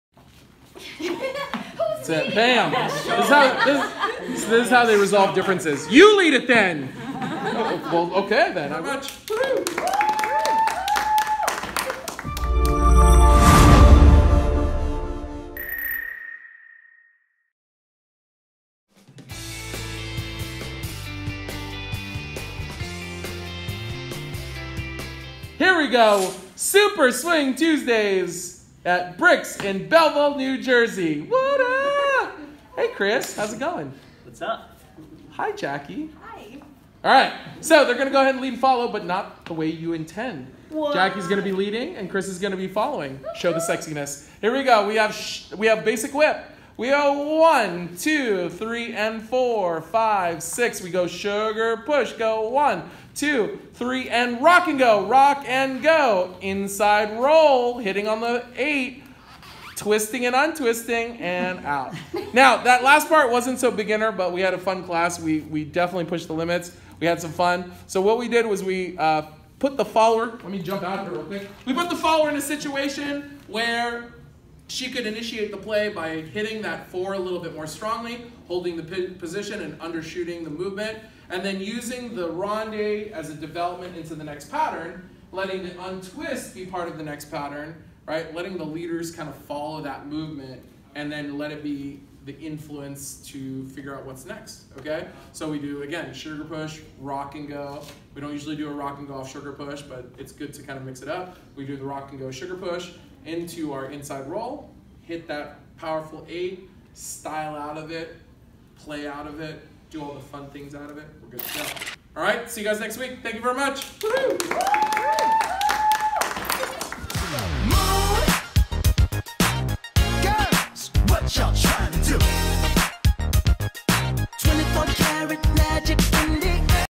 Super Swing Tuesdays Beg Class w/Arjay Centeno WCS @Brix October 17
October 17 West Coast Swing Class Video Recap

Drop in Classes every Tuesday in Belleville NJ at Brix

Because this is a drop in class, I teach this class to focus on movement and simple basic principles of partner dancing for West Coast Swing. More detailed instruction can be followed up with contacting the instructor for private instruction or doing follow up instruction with the instructor's DVDs available for purchase on arjaycenteno.com

Learning note*
In this class I constantly try to push the students in understanding a piece of my teaching philosophy within cool patterns and styling.

Keep in mind the dance of West Coast Swing is a versatile and ever changing dance, so the patterns, styling and teaching style in this video is my personal preference.